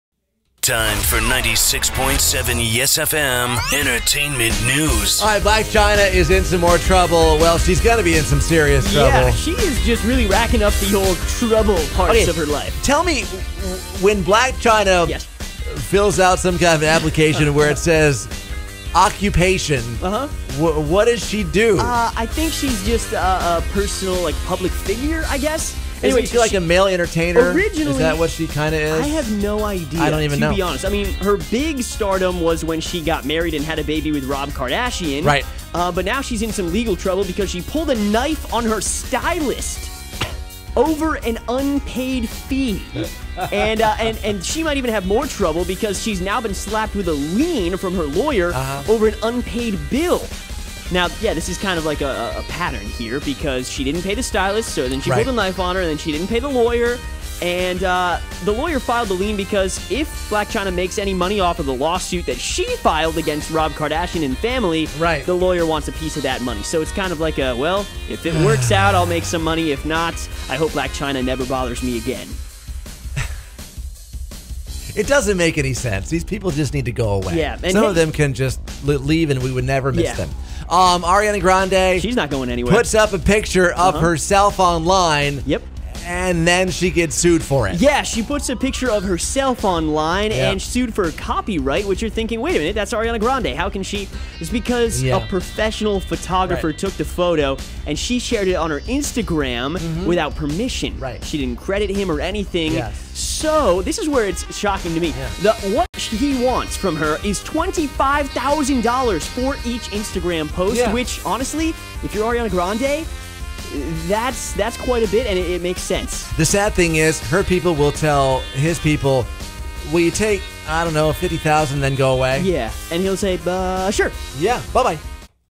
Blac Chyna Troubles & Ariana Grande Gets Sued
There’s more legal trouble for BLAC CHYNA. After being accused of pulling a knife on her stylist over an unpaid fee, Chyna has been slapped with a lien by her lawyer over an unpaid bill. While it would seem a pattern is developing with Chyna, attorney Lisa Bloom filed the lien to make sure she gets paid if Chyna wins any money from Rob Kardashian or his family in her ongoing lawsuit.

ARIANA GRANDE is being sued for posting photos of herself online. Barbera is seeking up to $25,000 for each violation, plus punitive damages.